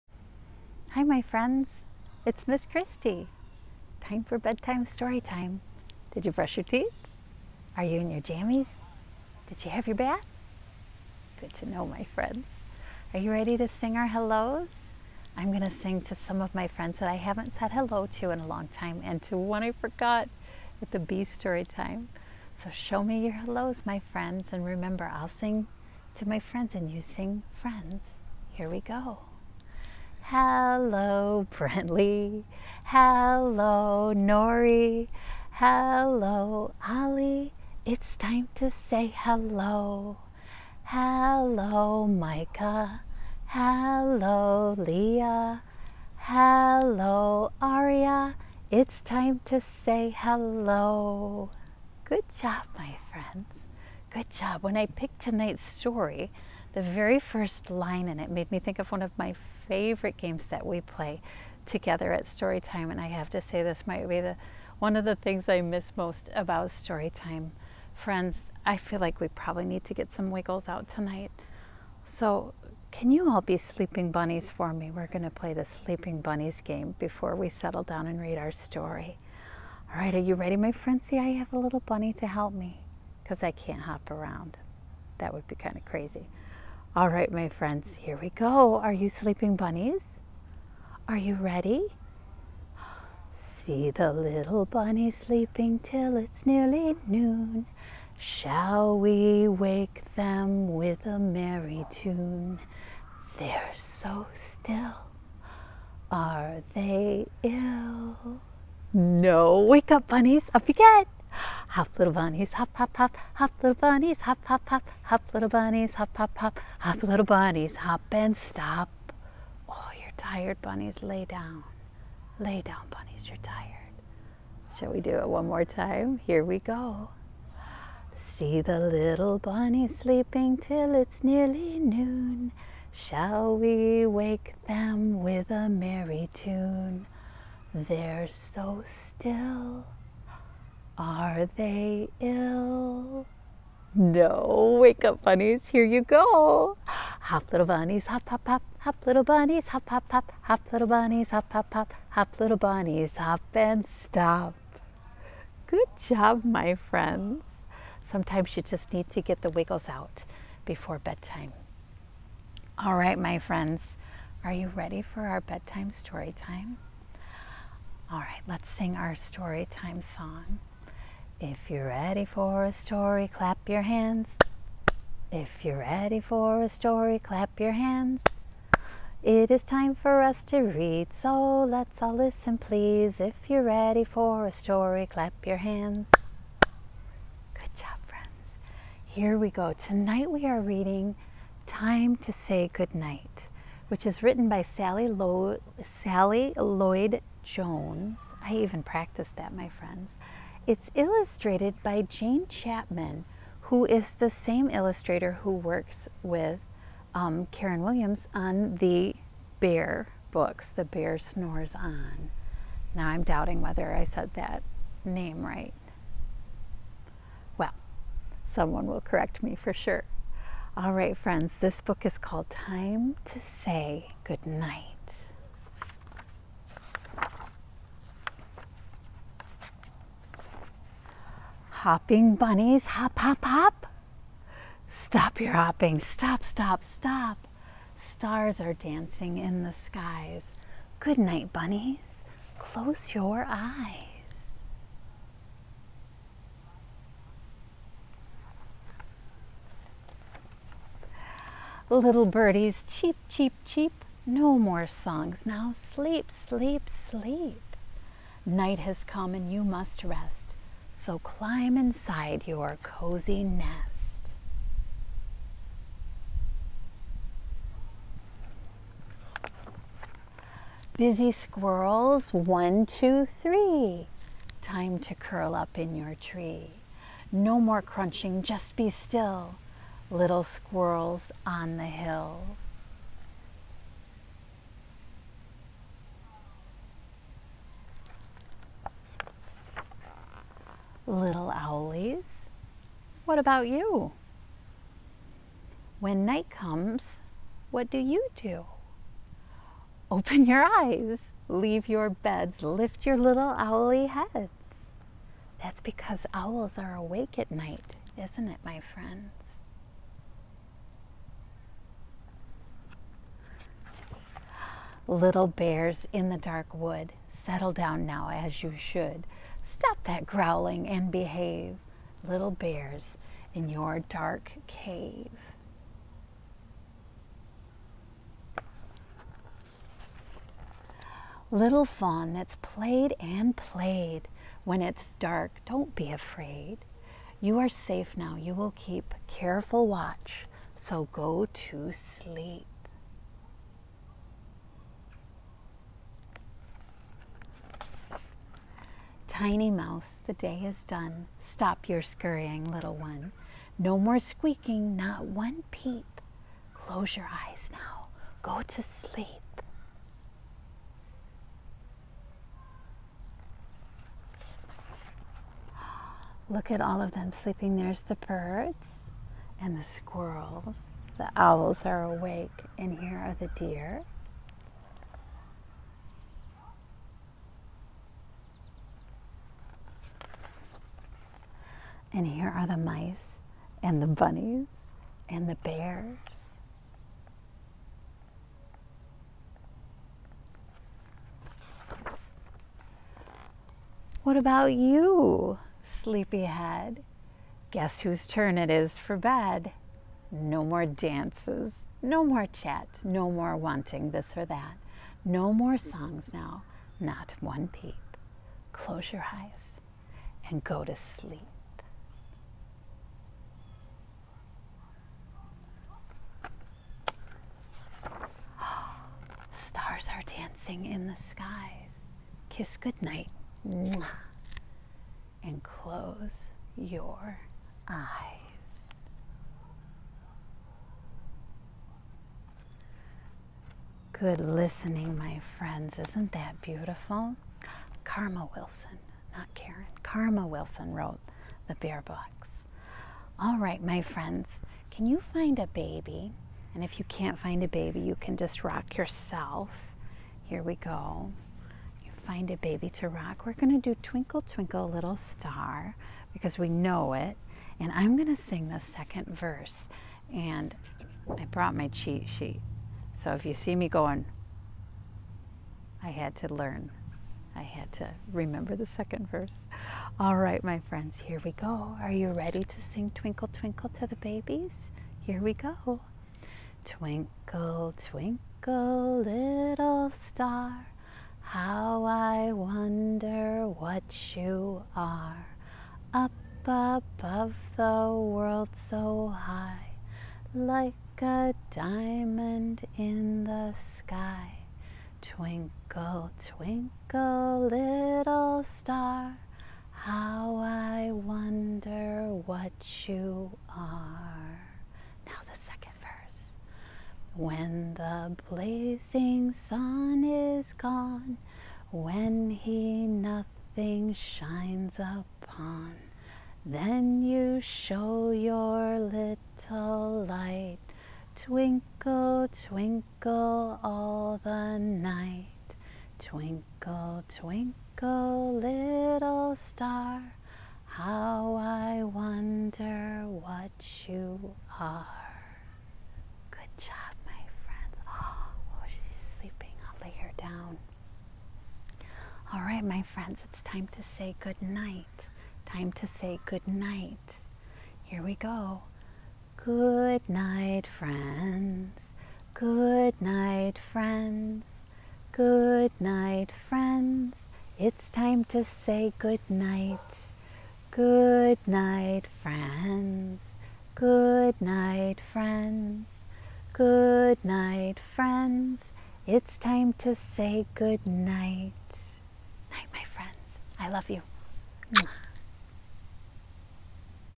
Bedtime Storytime with Ms. Kristy // Time to Say Goodnight
Join us for sleepy bunnies and birdies and bears as we read "Time to Say Goodnight", written by Sally Lloyd-Jones and illustrated by Jane Chapman, published by Harper Collins.